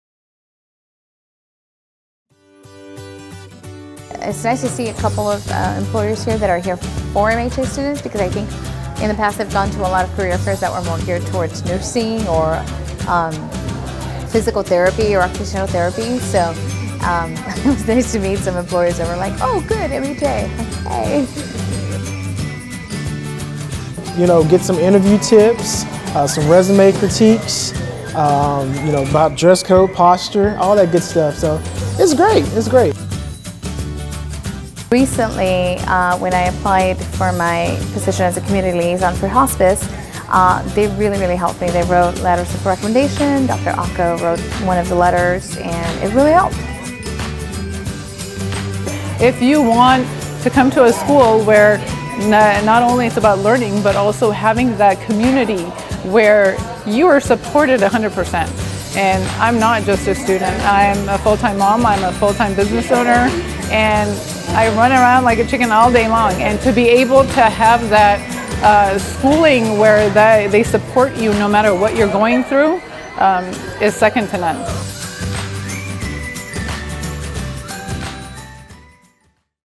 WCU Career Builder: Graduate Studies Hosts First Coffee and Careers
WCU is committed to student success both in the classrooms and in the job market.